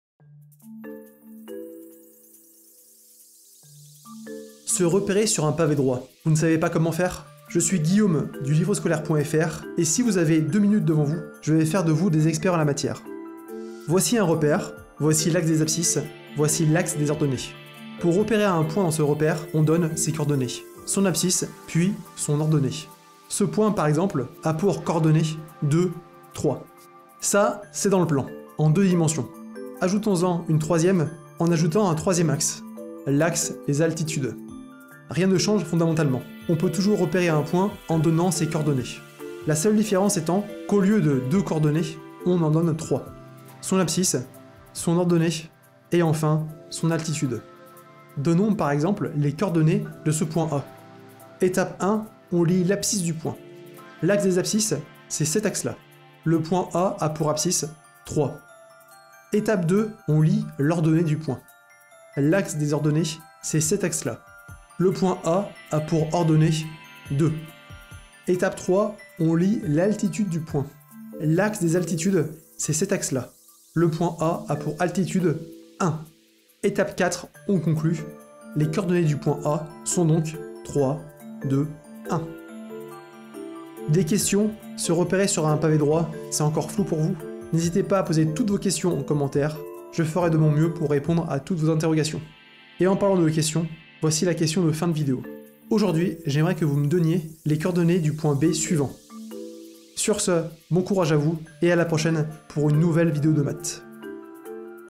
Se repérer sur un pavé droit
Se repérer sur un pavé droit tu trouves ça compliqué ? Pas de panique, si tu as quelques minutes devant toi, je vais faire de toi un expert en la matière.

Des questions ? N’hésite pas à les poser en commentaire.
N’oublie pas non plus de répondre en commentaire à “La question de la fin”.

Plan de cette méthode :
00:00 : Intro
00:13 Explications
00:48 Énoncé
00:51 Étape 1 : Lire l'abscisse du point
00:59 Étape 2 : Lire l'ordonnée du point
01:09 Étape 3 : Lire l'altitude du point
01:16 Étape 4 : Conclure
01:31 La question de la fin